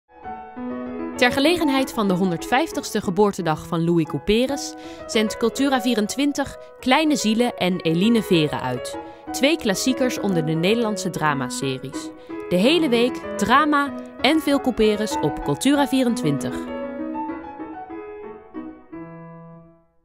THEMA: 150 jaar Louis Couperus
De Stille Kracht, Eline Vere, De boeken der kleine zielen, Noodlot, Extaze en Antiek Toerisme, het zijn enkele titels uit het gigantische oeuvre van schrijver Louis Couperus. Dit jaar is het 150 jaar geleden dat hij geboren werd. Hij stierf 90 jaar geleden. Cultura24 staat stil bij misschien wel de grootste, maar meest ongelezen schrijver van Nederland. Van maandag 7 t/m zaterdag 12 oktober, iedere avond vanaf 20:30 uur.